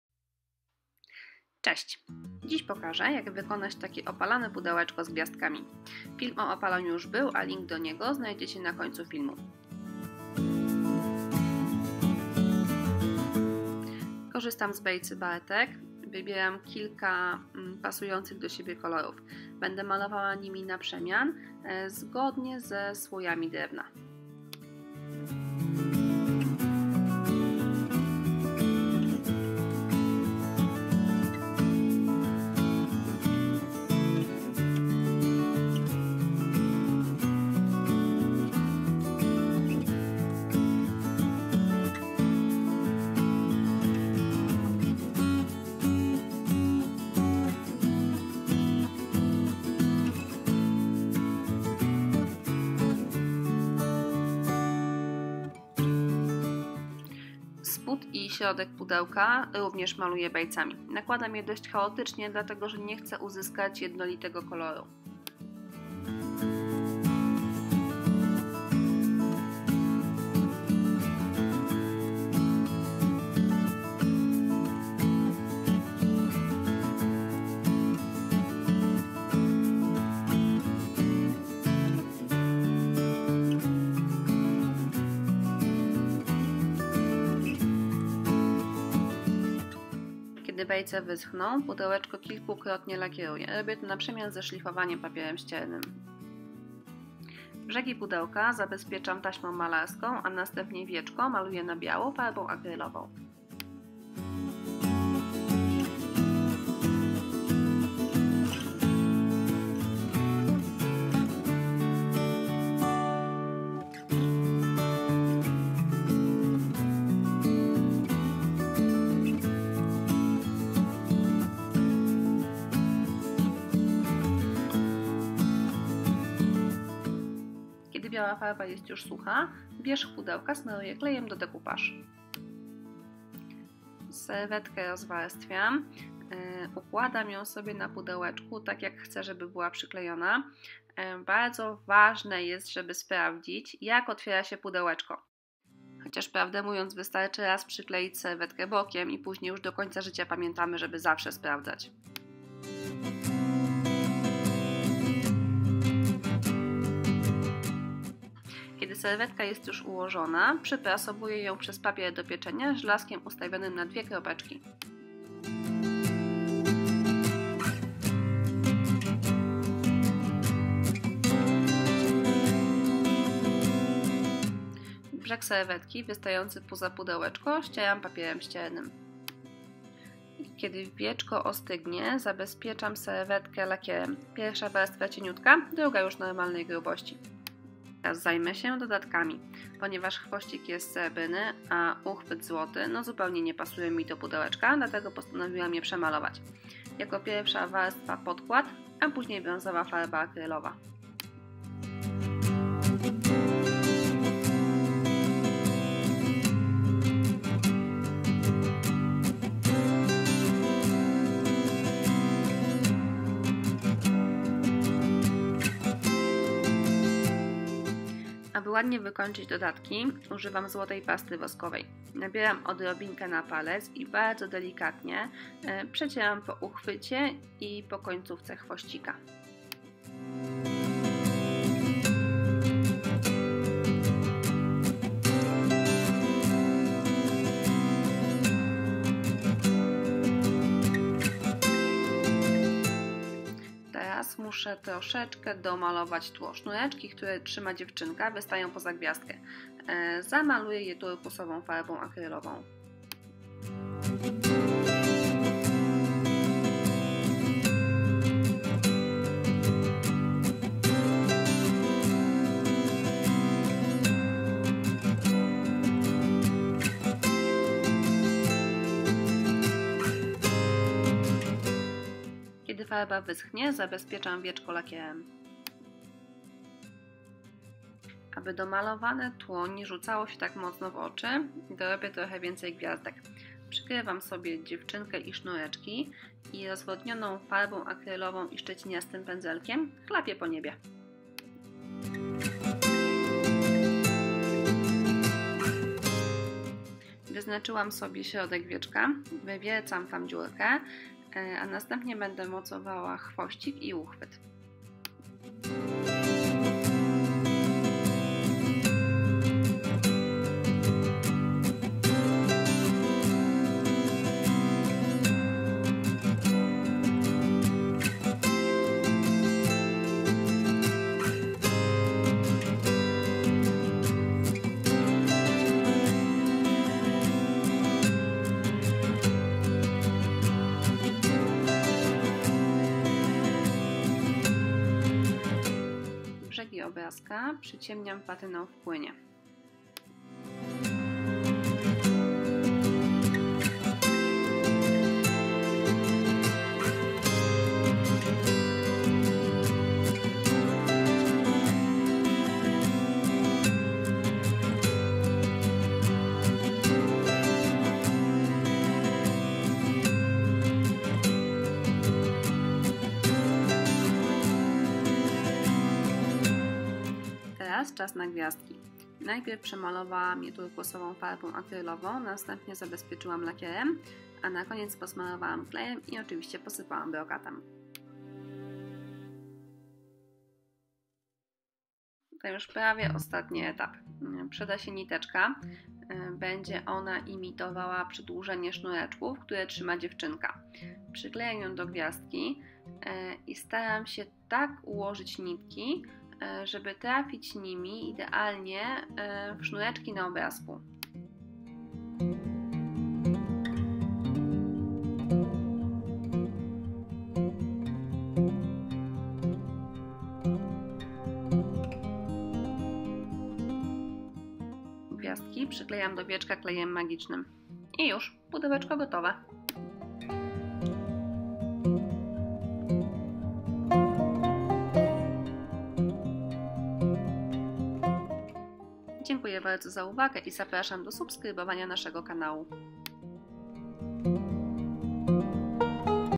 Szkatułka decoupage - pudełko z gwiazdkami - tutorial DIY
Szkatułka decoupage -  pudełko z gwiazdkami - film przedstawia jak wykonać pudełeczko z gwiazdkami w technice decoupage.